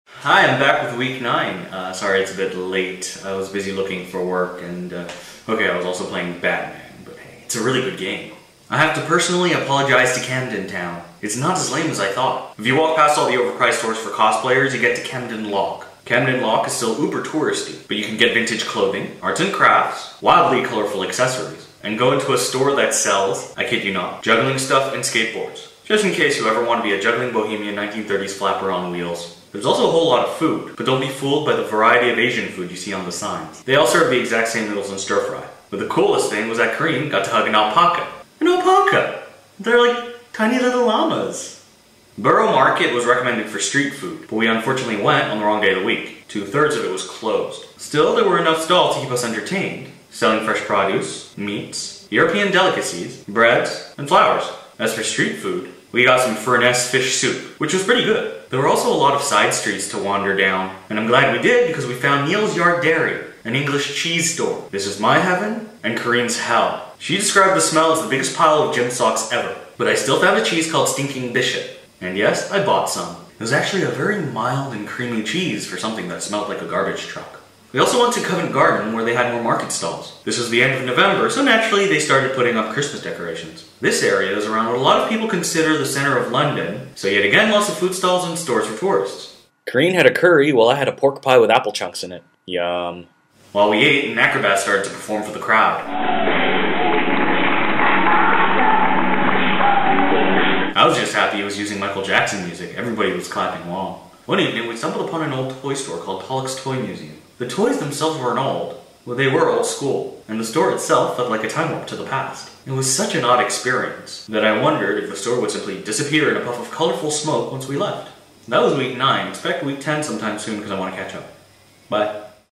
A year in London - Week 9
My weekly-ish video blog about life in London. I cover Camden Lock, Borough Market, Covent Garden and Pollock's Toy Store.